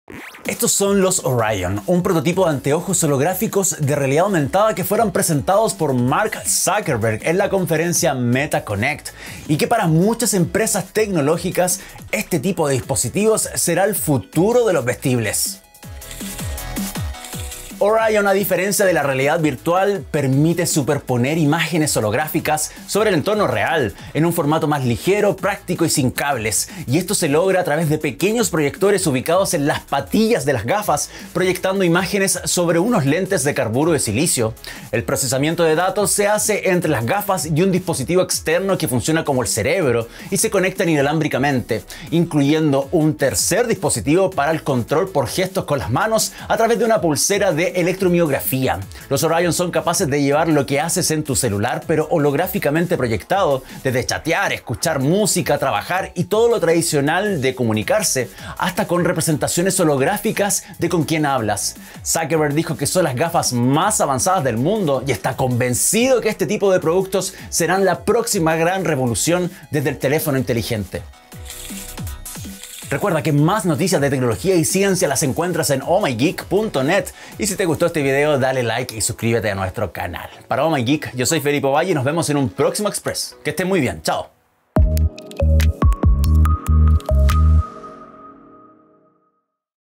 «Orion» son las nuevas gafas holográficas de realidad aumentada de Meta
Meta ha presentado su prototipo de gafas «Orion», un dispositivo promete transformar la manera en que los usuarios interactúan con el mundo, al ofrecer una experiencia holográfica completa.

Orion superpone imágenes holográficas sobre el entorno real, todo en un formato mucho más ligero, práctico y sin cables. Siendo un proyecto que lleva años en desarrollo, y según la compañía, representan el par de gafas ‘AR’ más avanzado jamás creado.

Los hologramas se muestran a través de pequeños proyectores ubicados en las patillas de las gafas, proyectando imágenes sobre unas lentes de carburo de silicio. A esto se suma un campo de visión de 70 grados, lo que mejora notablemente la percepción del entorno.

Otra de las innovaciones es el uso de una pulsera de electromiografía (EMG), que se utiliza para el control por gestos con tus manos. De igual forma, Orion puede ser utilizado por comandos de voz y seguimiento de ojos.

La compañía anunció que aunque este producto aún está en una fase de prototipo, no es únicamente un experimento de laboratorio, sino un paso tangible hacia un producto comercial en el futuro.